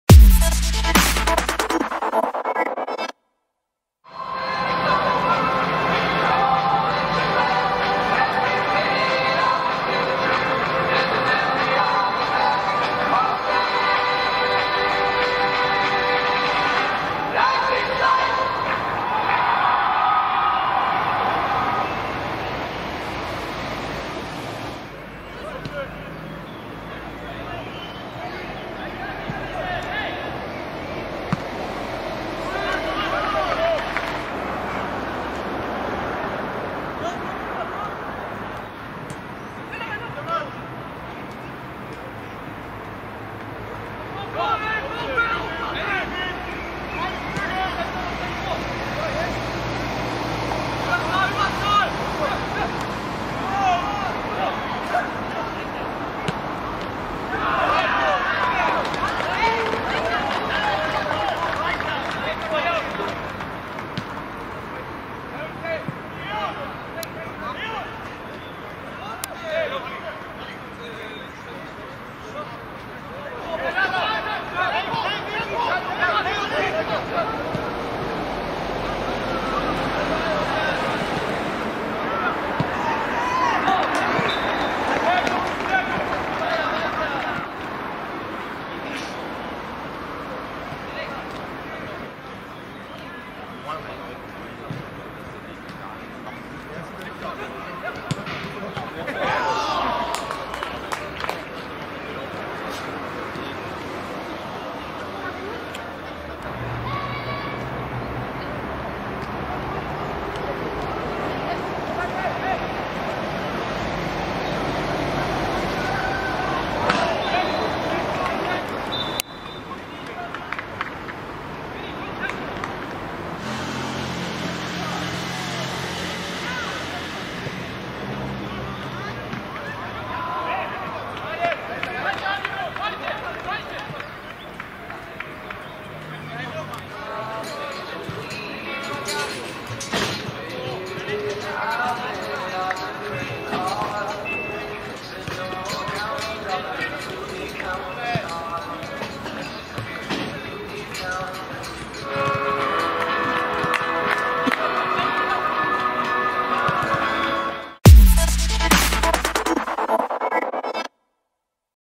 Bayer 04 Leverkusen U15 - VfL Wolfsburg U15 (Vorrunde, Bernesto Champions Cup)
Vorrunde (19.05.18)

Tore:
1:0 Emrehan Gedikli

Kader Bayer 04 Leverkusen U15:
1 Jakob Brambach, 2 Leon Pesch, 3 Ali Barak, 4 Christian Schwieren, 5 Oliver Fobassam, 6 Joshua Eze, 7 Felix Erken, 8 Okan Keskin, 9 Aaron Tshiumuanga, 19 Emrehan Gedikli, 11 Luca Wesemann, 12 Jannis Fissl, 13 Demas Podelov, 14 Simon Ludwig, 15 Fabio Schmidt, 16 Justin Urrigshardt

Kader VfL Wolfsburg U15:
1 Philipp Schulze, 3 Maik Pörner, 6 Marcel Reim, 8 Asil Haider Ismail, 9 Eliaz Rahimic, 10 Marlo Moretti, 11 Fabio Di-Michele-Sanchez, 12 Moritz Berg, 13 Jaime Marques Oliveira, 14 Robin Luschert, 15 Willi Reincke, 16 Lucien Littbarski, 17 Josue Tortora, 18 Malte Istock, 21 Jona Renner